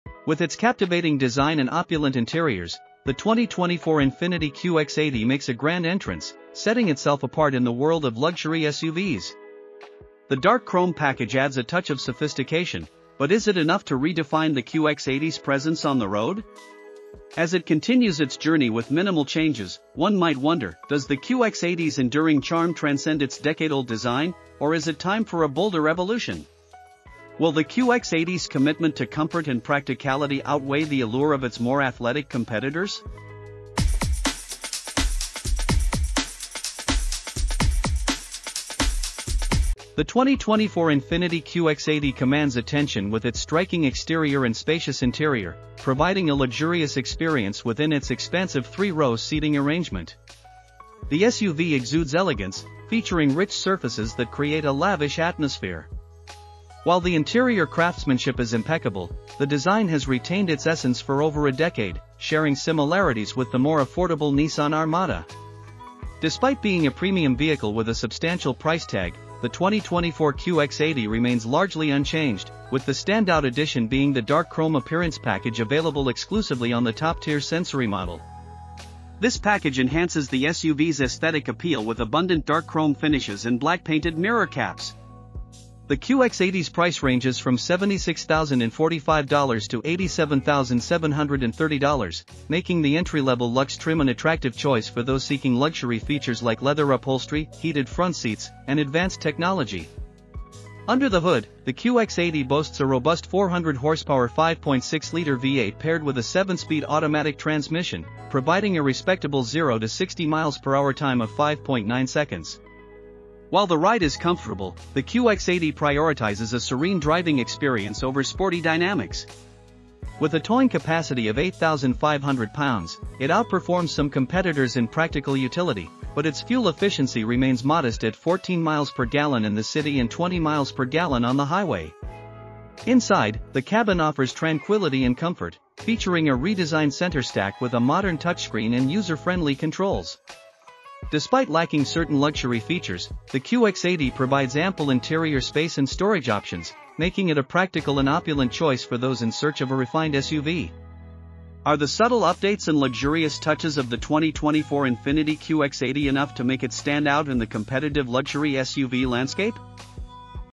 What's Inside the 2024 INFINITI QX80? A Closer Look at Opulence!
Step into a realm of sophistication with the all-new 2024 Infiniti QX80, where design meets luxury in perfect harmony. This year, the QX80 makes a stunning entrance with its captivating redesign, featuring the exclusive Dark Chrome package that adds an extra layer of elegance to its already handsome bodywork. Immerse yourself in acres of rich-looking surfaces and indulge in the spacious three-row interior, carefully crafted to provide both comfort and opulence.

Inside the QX80, experience the perfect blend of timeless design and modern touches. The redesigned center stack accommodates a state-of-the-art touchscreen and user-friendly controls, while the HVAC system enjoys a new layout with physical knobs for dual climate control. With standard leather upholstery, heated front seats, and a 13-speaker Bose stereo system, the QX80 pampers you at every turn. Join us as we explore the impeccable craftsmanship, powerful engine options, and captivating design of the 2024 Infiniti QX80. Are you ready to redefine your journey in the lap of luxury?

Embark on an exploration of the QX80's remarkable features, from its engine prowess to the carefully designed interiors that make every ride feel like a royal jaunt. Delve into the details of its redesign, discover the thoughtfully incorporated Dark Chrome elements, and find out how this luxury SUV is setting new standards for elegance. Join us in this detailed review of the 2024 Infiniti QX80, where we unravel the secrets behind its captivating design, impressive performance, and the ultimate comfort it offers. Is the QX80 your ticket to a world where luxury knows no bounds? Let's find out together.